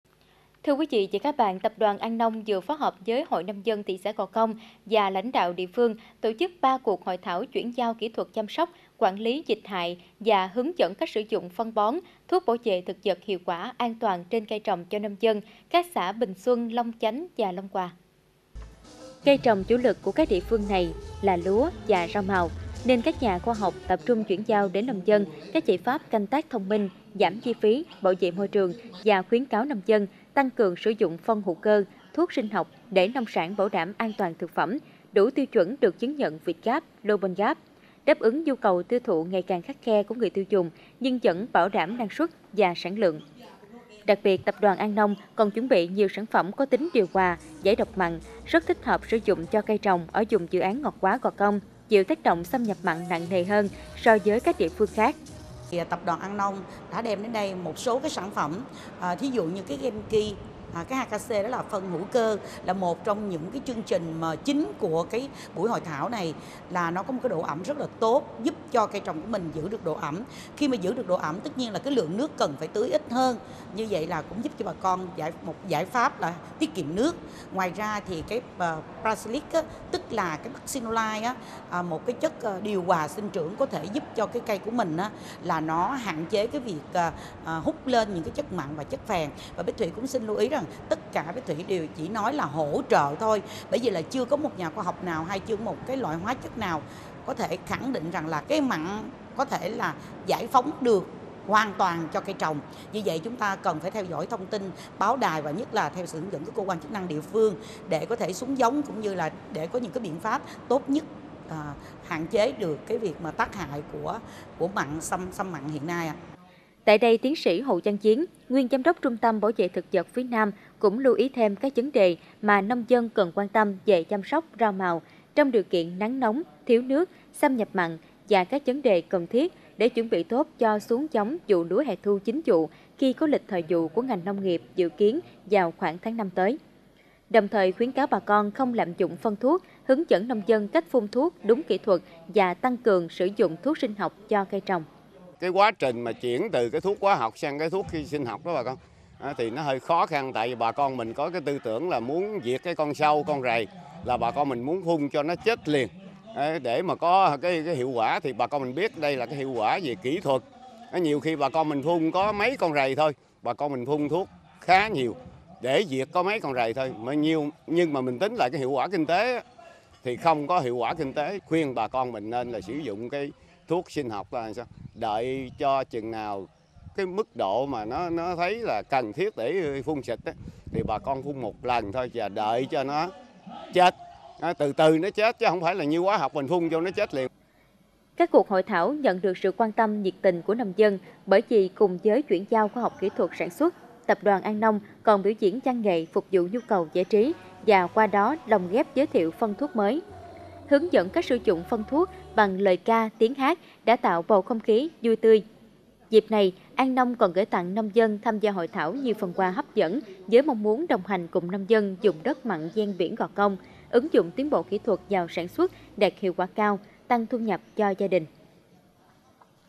Tập đoàn An Nông hội thảo cùng hội nông dân gò công tập huấn kỹ thuật cho cây trồng
🔰 THTG - Tập đoàn An Nông cùng Hội Nông dân các địa phương tập huấn kỹ thuật, tổ chức hội thảo giúp nông dân sử dụng phù hợp, hiệu quả và an toàn trên từng loại cây trồng, trên từng vùng đất canh tác ở các xã thuộc vùng dự án ngọt hóa Gò Công nhằm chuyển giao kỹ thuật mới về chăm sóc cây trồng mùa hạn mặn, đồng hành cùng nông dân trên hành trình sản xuất thích ứng với biến đổi khí hậu và góp phần cùng ngành nông nghiệp đạt mục tiêu giảm chi phí sản xuất, đạt nông sản sạch, bảo vệ môi trường.( THTG )
🏢 TẬP ĐOÀN AN NÔNG - 𝗔𝗡𝗡𝗢𝗡𝗚 𝗚𝗥𝗢𝗨𝗣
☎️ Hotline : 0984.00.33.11
♻️ An Nông Chung Sức Cùng Nhà Nông